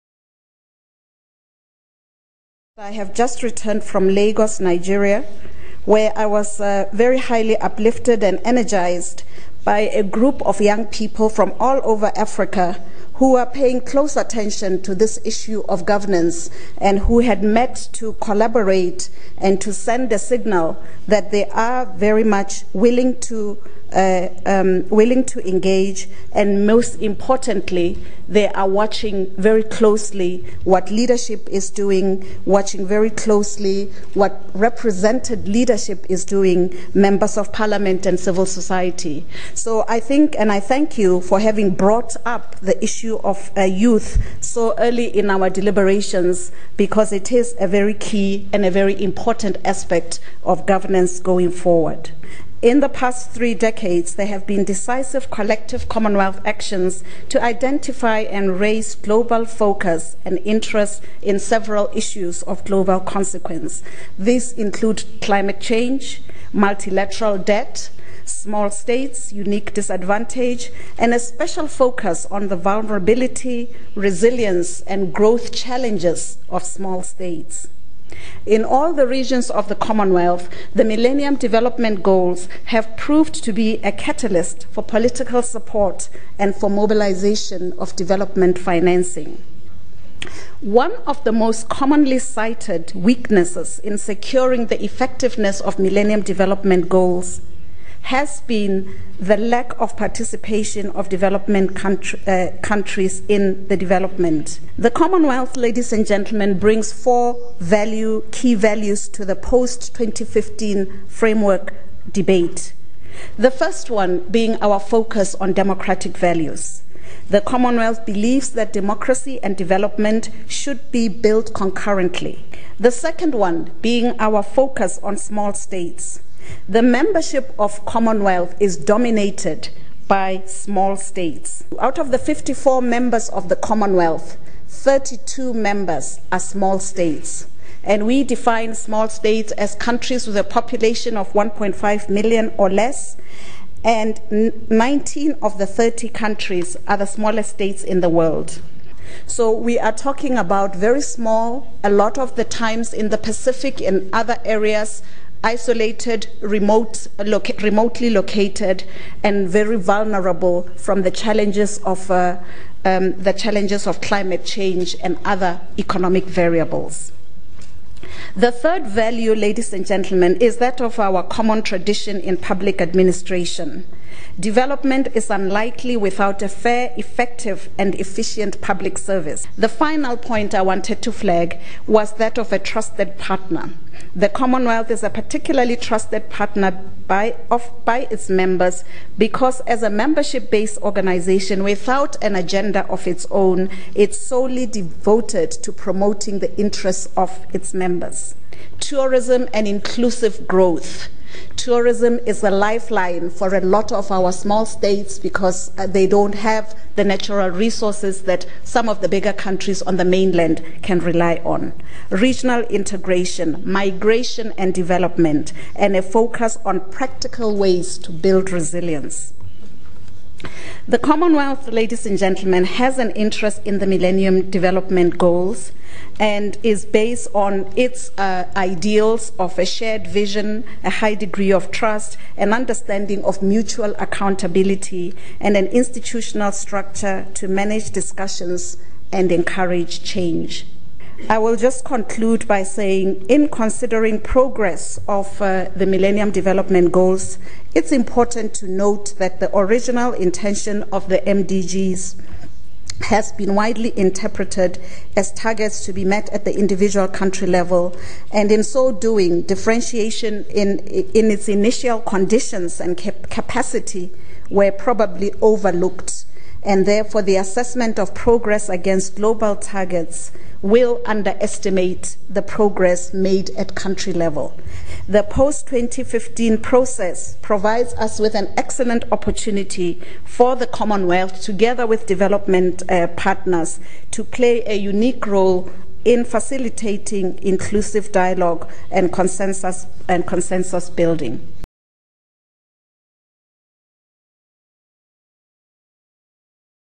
Address by Mmasekgoa Masire-Mwamba, Commonwealth
African Regional Dialogue - The New Development Agenda: Post-2015 Global Thematic Consultation on Governance, 11-12 October 2012, Johannesburg, South Africa

Address by Mmasekgoa Masire-Mwamba, Deputy Secretary-General, Commonwealth Organization